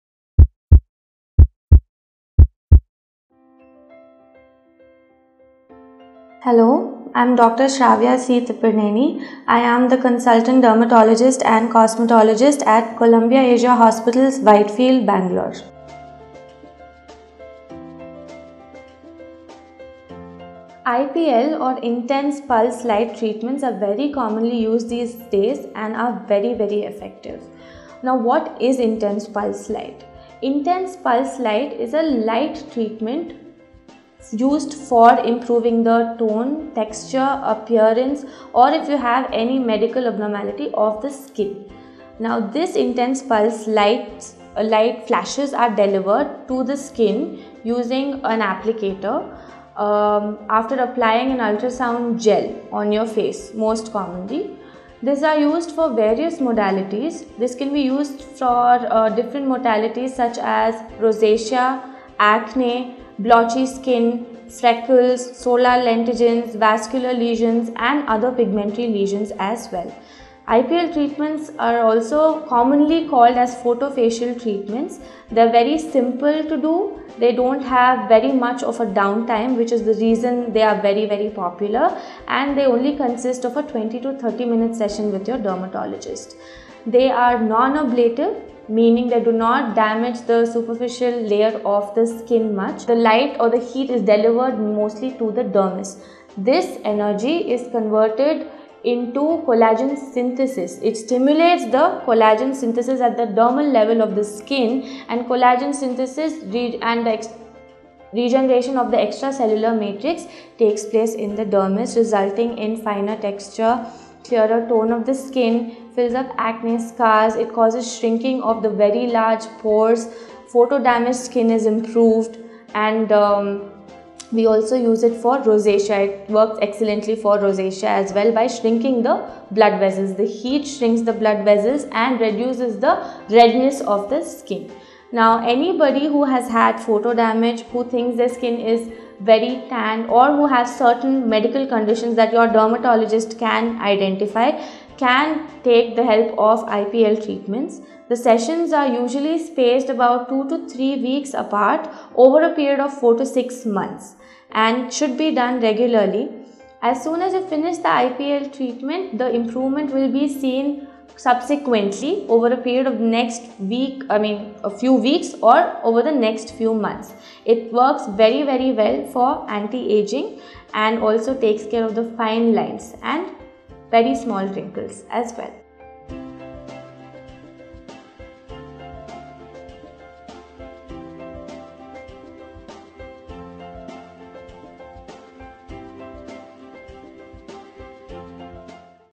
What to expect from IPL Photofacial Treatment? Are they effective?-Dr. Sravya Tipirneni
IPL or Intense Pulse Light Treatments are very commonly used these days and are very effective. Now what is Intense Pulse Light is a light treatment is used for improving the tone, texture, appearance or if you have any medical abnormality of the skin. This Intense Pulse Light, the light flashes are delivered the skin using an applicator after applying an ultrasound gel on your face most commonly. These are used for various modalities. This can be used for different modalities such as rosacea, acne, blotchy skin, freckles, solar lentigines, vascular lesions and other pigmented lesions as well. IPL treatments are also commonly called as photofacial treatments. They are very simple to do. They don’t have very much of a down time, which is the reason why they are very very popular and they only consist of a 20 to 30 minute session with your dermatologist. They are non-ablative, meaning they do not damage the superficial layer of the skin much. The light or the treatment is delivered mostly to the dermis. This energy is converted into collagen synthesis. It stimulates the collagen at the dermal layer of the skin and collagen synthesis and regeneration of the extracellular matrix takes place in the dermis, resulting in finer texture, clearer tone of the skin, fills up acne scars, causes shrinking of the very large pores, photodamage skin is improved and we also use it for rosacea. It works excellently for rosacea by shrinking the blood vessels, the heat shrinks the blood vessels and reduces the redness of the skin. Now anybody who has had photodamage, who thinks that their skin is very tanned or who has certain medical conditions that your dermatologist can identify can take the help of IPL treatments. The sessions are usually spaced about 2 to 3 weeks apart, over a period of 4 to 6 months and should be den regularly. As soon as you finish the IPL treatment, the improvement will be seen subsequently over a period of next week, a few weeks or the next few months. It works very very well for antiageing and also takes care of fine lines and very small wrinkles as well.